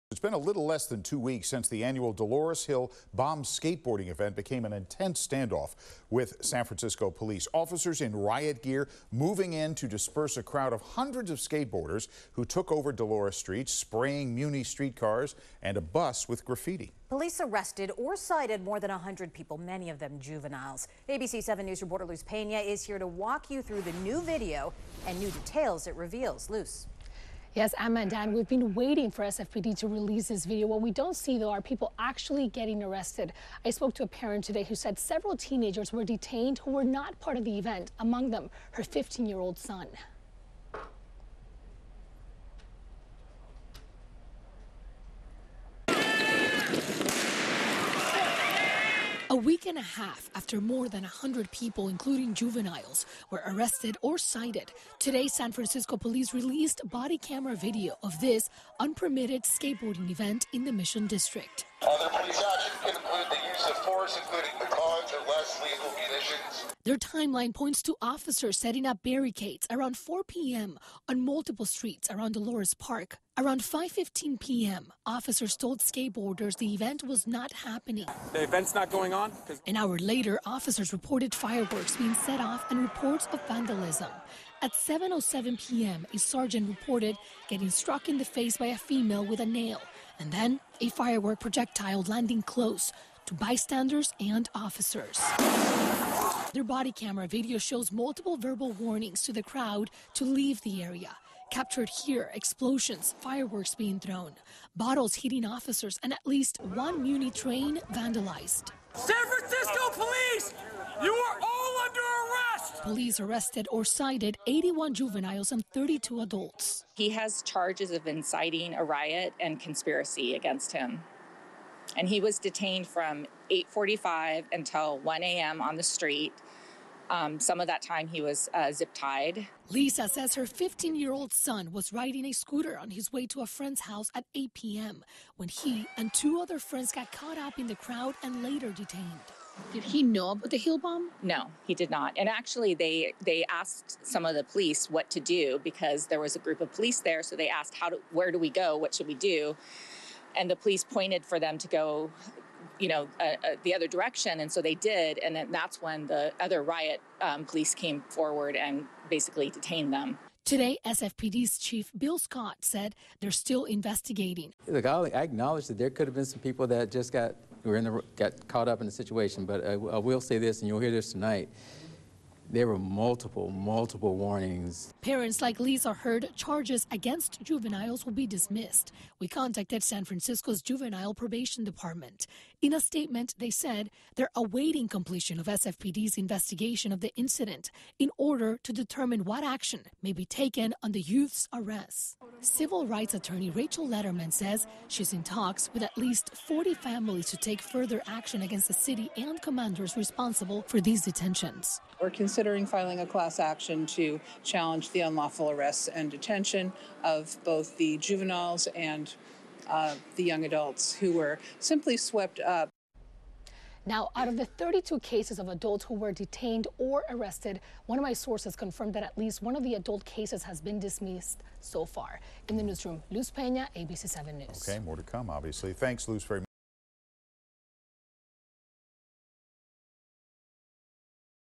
Bodycam video shows moments before 100+ arrested, cited at SF event
After more than 100 people including juveniles were arrested or cited, San Francisco police on Wednesday released body camera video of the unpermitted event in the Mission District.